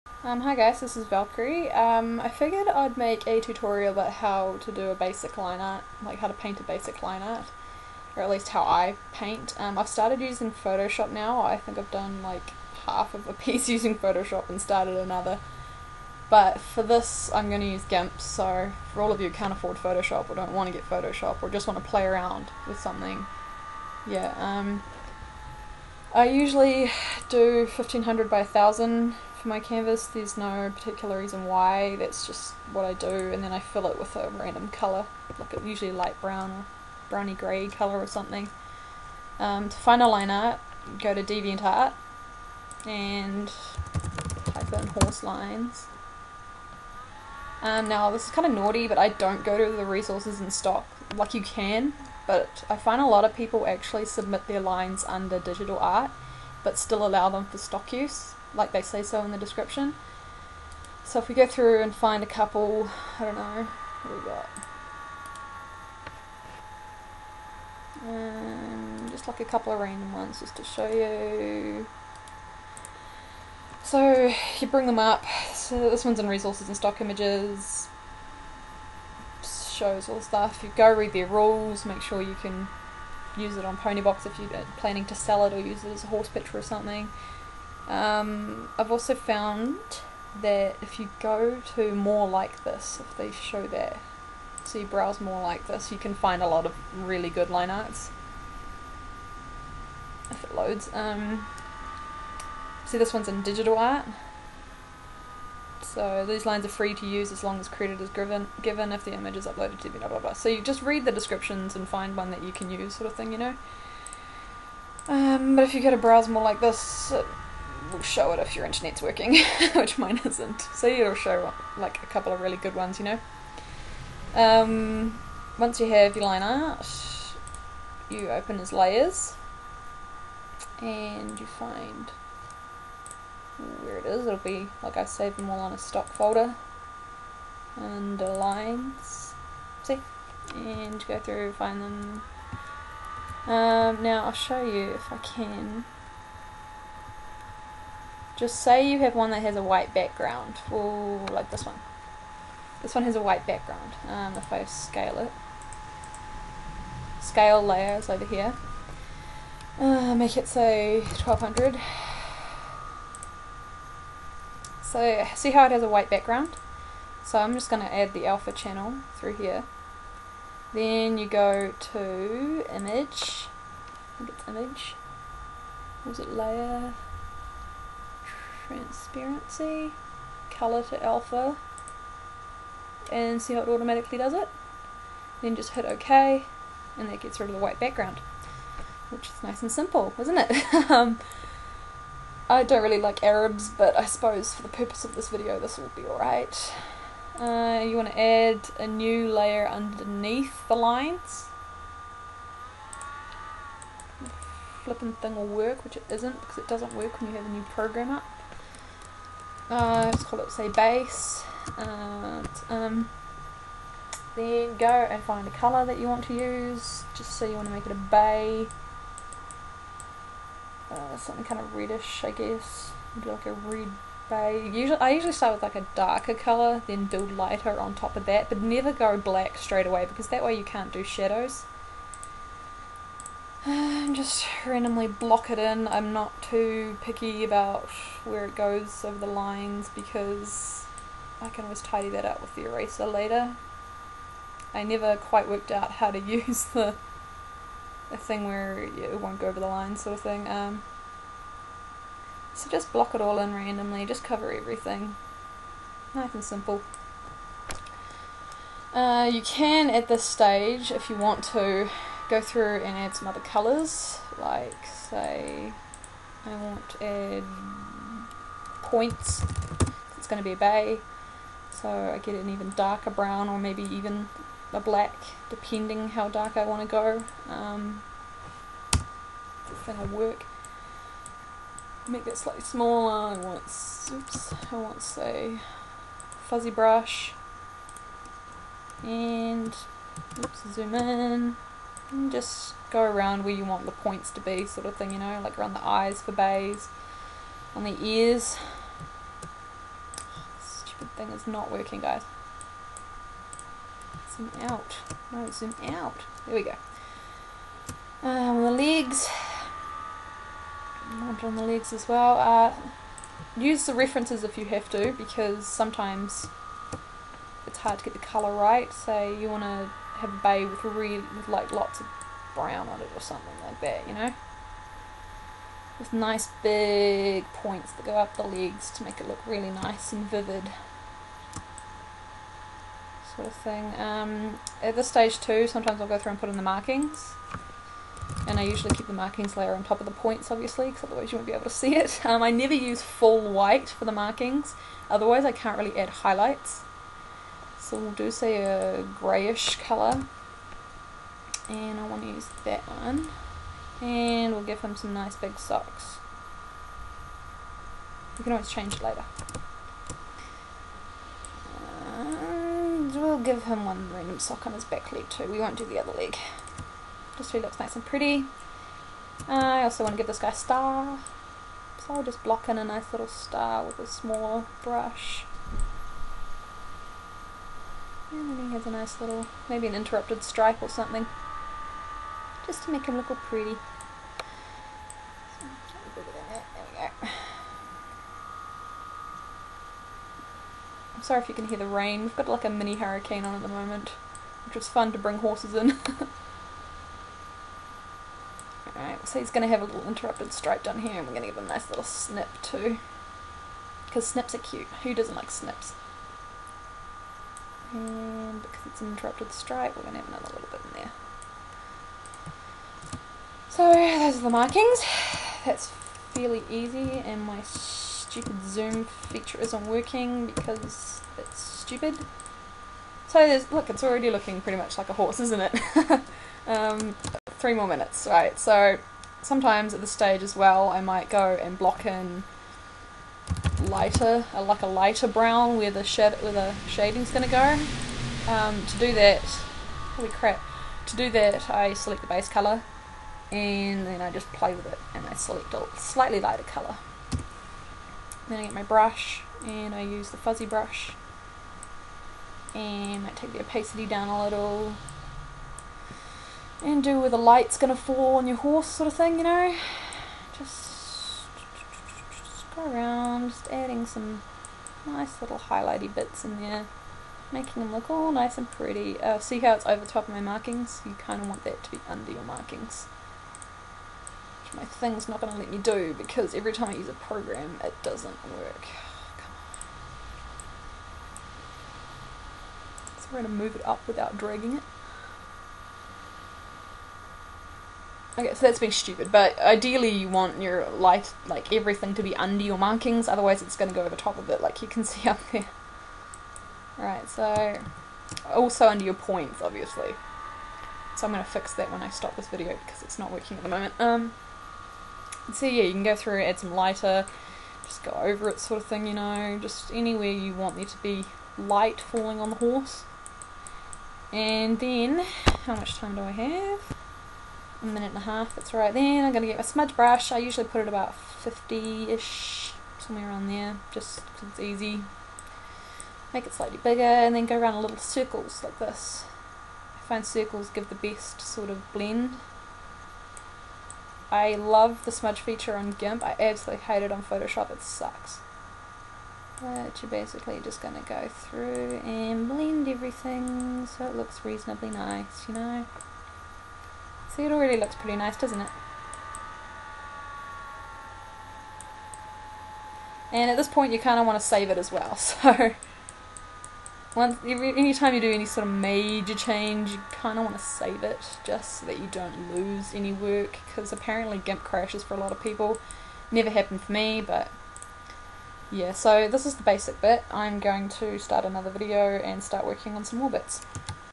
Valkyrie's Basic GIMP Lineart Tutorial PART 1
Some people in the past have asked me how I paint. Well, here it is!

Blocking and putting the basic colours on.